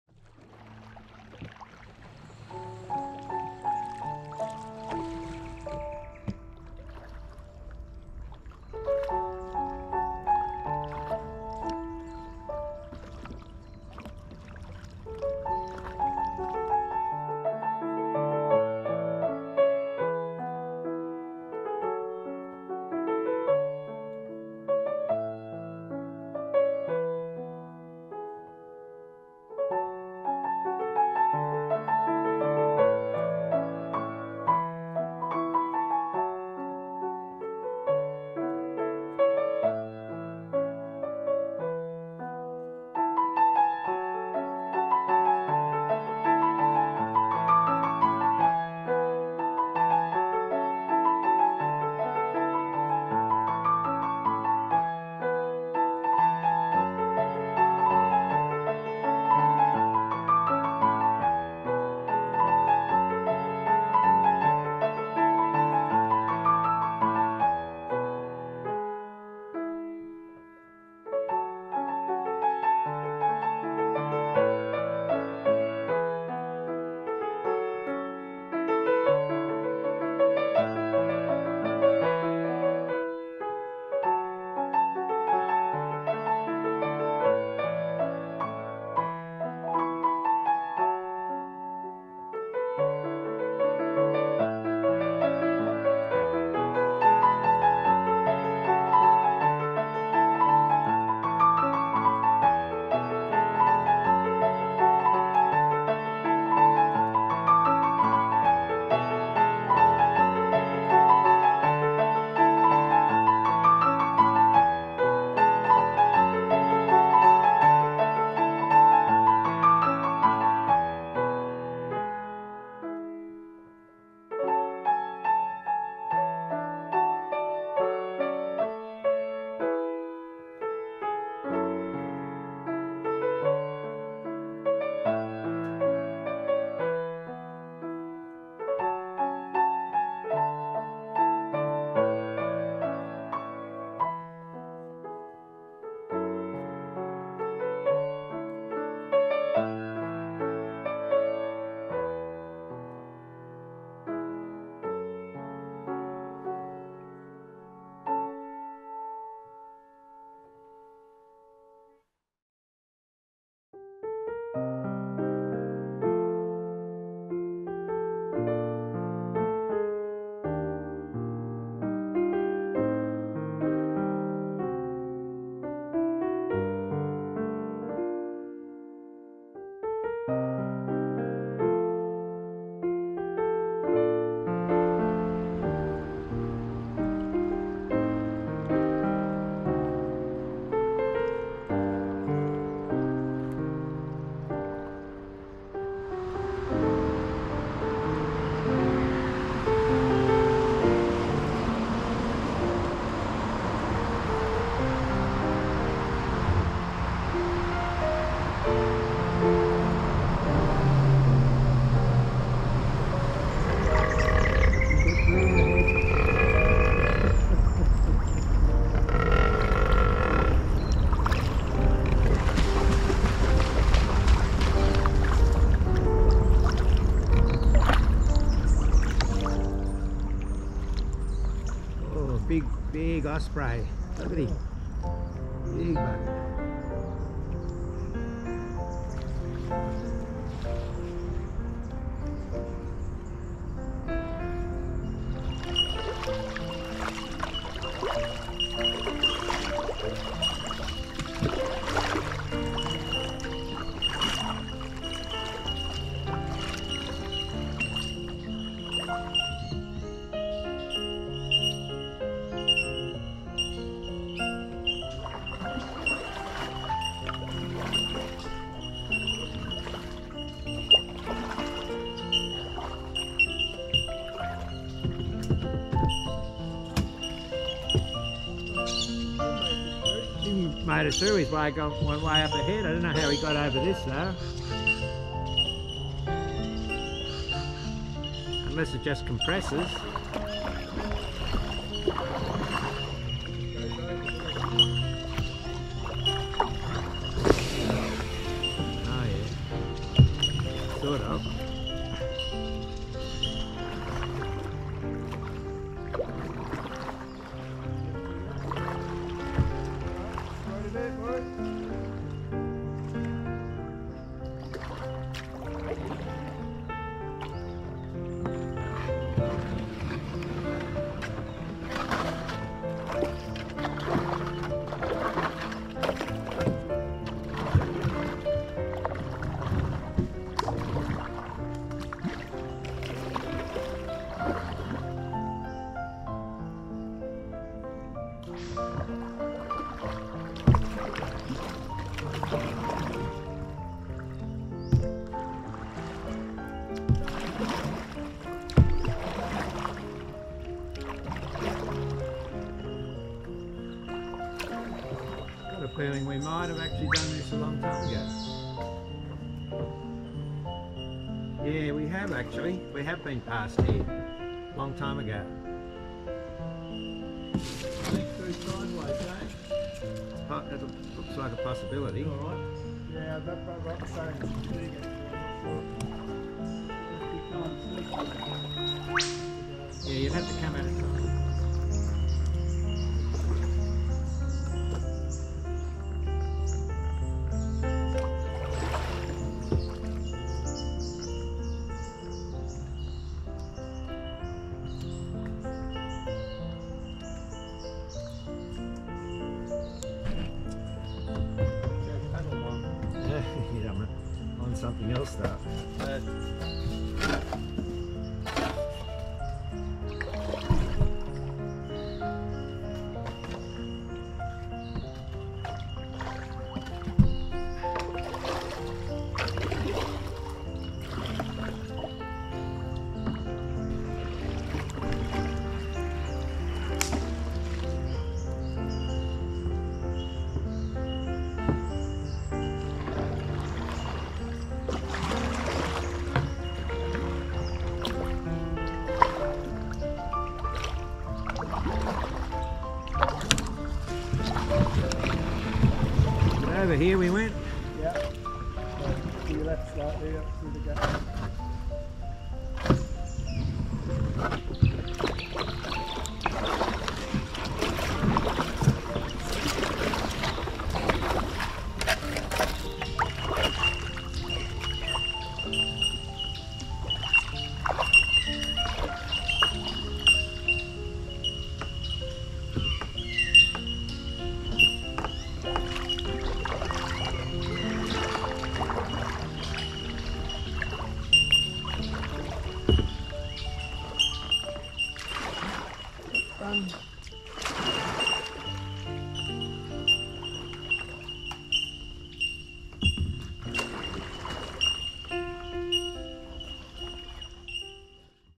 Wyong 18Apr24 8m
A paddle for about 5 Km upstream on Wyong Creek, from the Milk Factory to a log jam. The river has been infested with an invasive weed recently, especially around the pumping station.